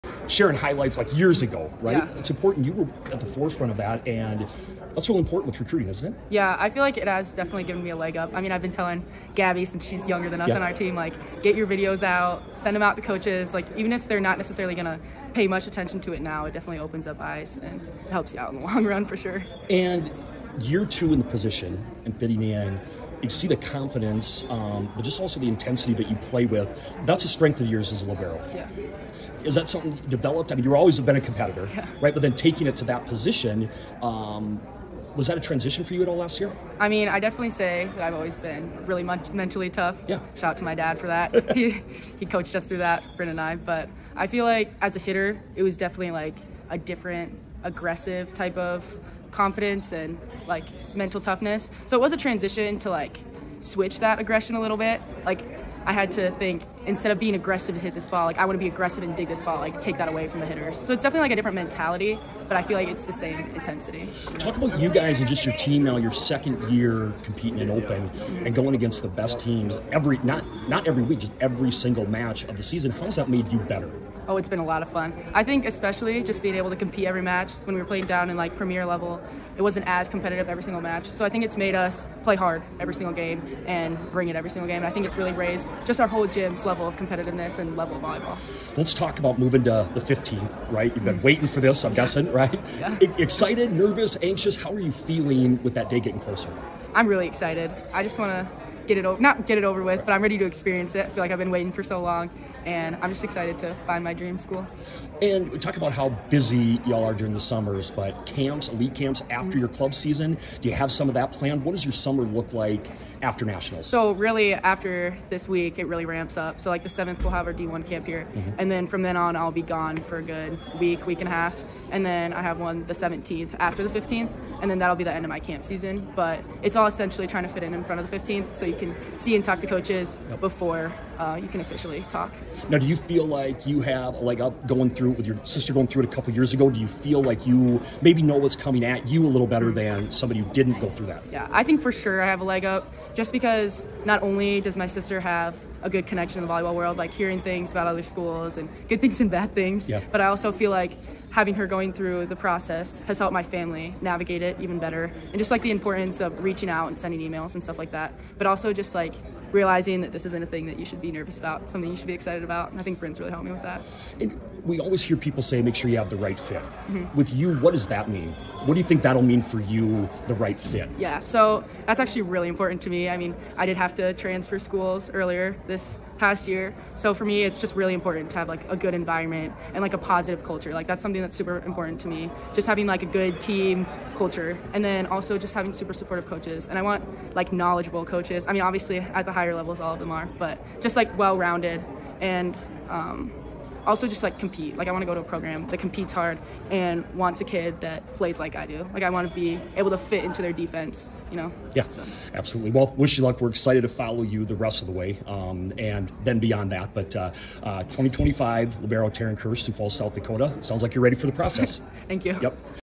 Kirsch talks June and excitement for the process
South Dakota 2025 Libero Brynn Kirsch talks about her upcoming June and how she's preparing for the her recruitment as she looks for her dream school.